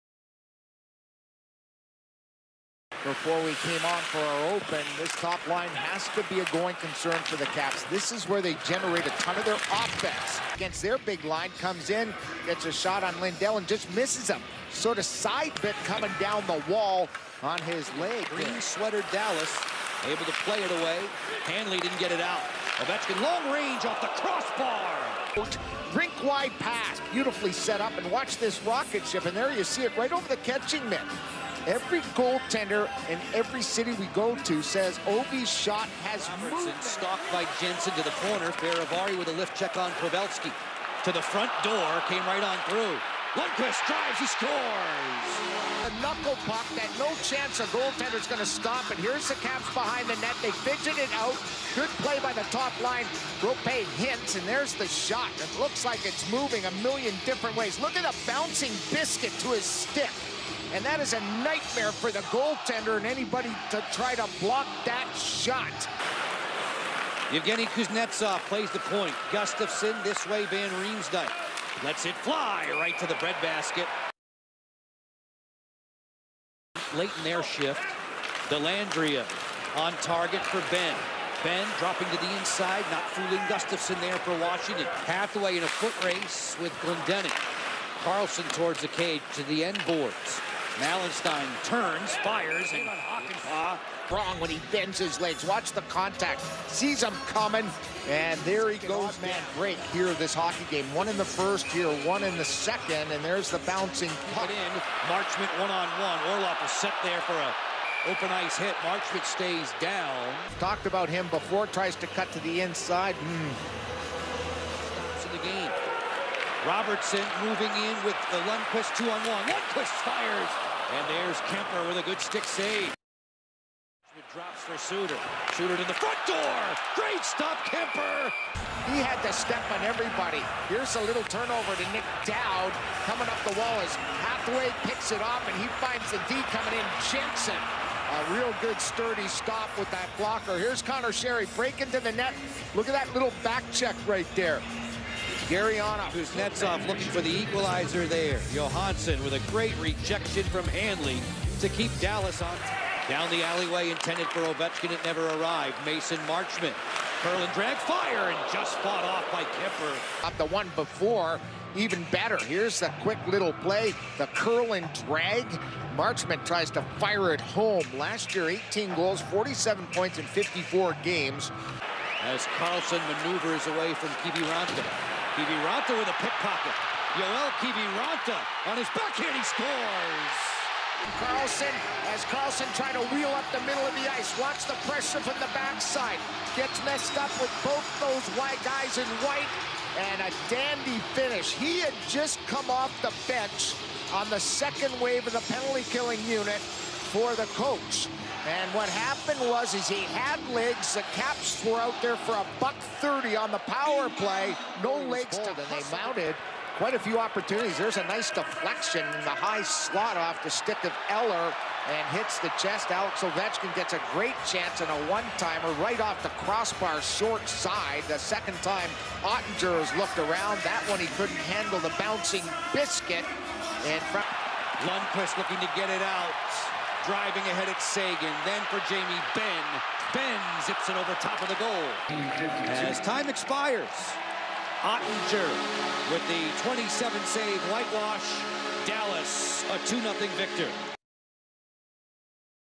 Washington Capitals [0:2] Dallas Stars. All goals in 5 min. The best moments.
"Dallas" beat "Washington" in the NHL regular season match — 2:0. "Stars" won the first victory after two consecutive defeats.